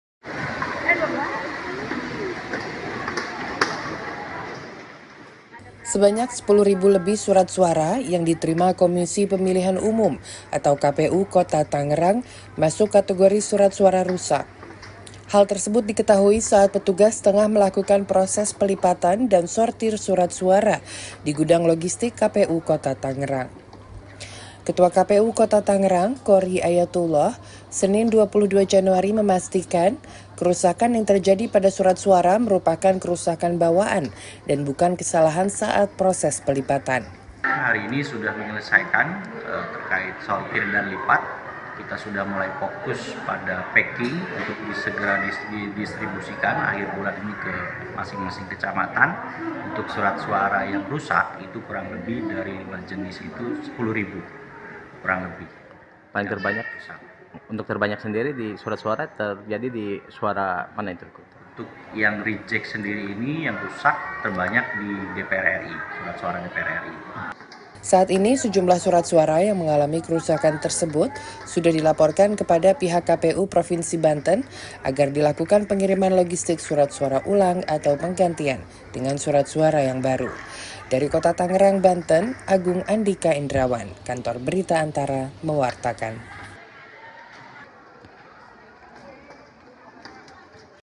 KPU Kota Tangerang dapati 10 ribu surat suara rusak
ANTARA - Komisi Pemilihan Umum (KPU) Kota Tangerang, Banten mencatat ada sebanyak 10 ribu lebih surat suara yang masuk kategori rusak. Ketua KPU Kota Tangerang, Qori Ayatullah pada Senin (22/1) menyebut pihaknya telah melapor ke KPU Provinsi Banten untuk meminta penggantian surat suara yang baru.(Agung Andhika Indrawan/Andi Bagasela/Ludmila Yusufin Diah Nastiti/Puji Lestari)